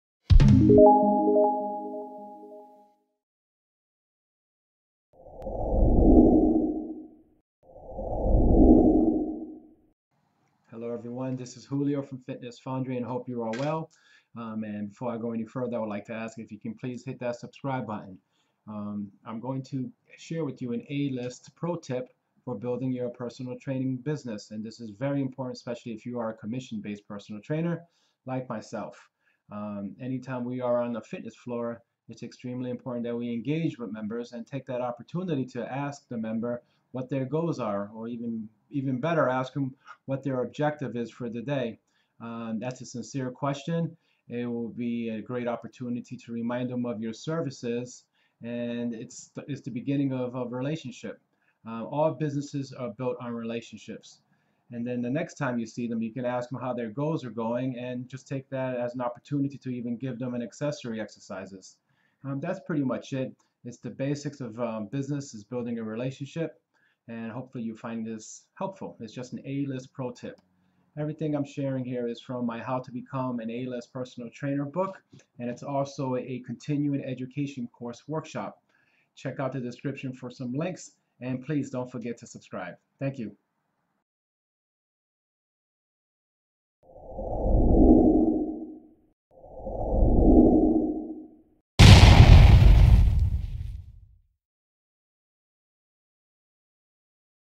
Personal Training Business Basics- Ask for Goals!
Quick video on the value of asking members for their goals and how it leads to building a personal training business a.k.a developing relationships.

Video is based on my CEU Approved Workshop and Book titled "How to Become an A-List Personal Trainer. Fundamentals For Optimal Service-Branding-Success".

Read my latest articles on health and fitness for the fitness pro

PS: Want to learn on "How to Become an A-List Personal Trainer"?